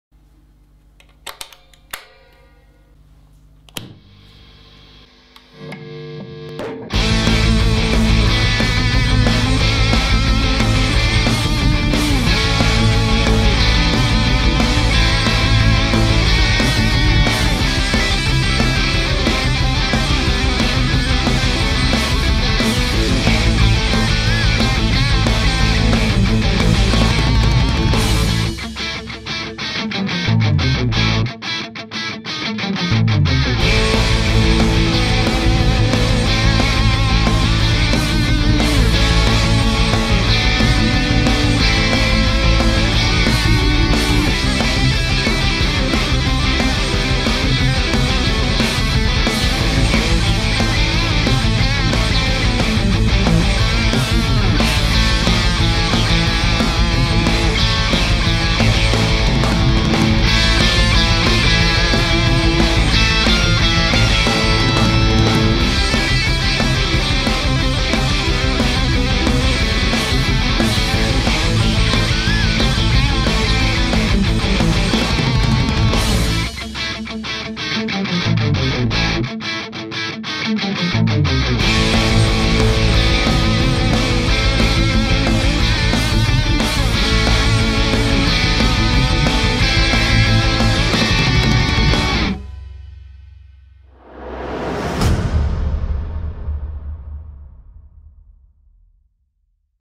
Marshall 2555X Silver Jubilee - Demo
Marshall 2555X Silver Jubilee aufgenommen von mit Marshall Cabinet MR 1936 und einer PRS Custom 24, abgenommen mit einem Shure SM57 und einem Beyerdynamic M 69 TG.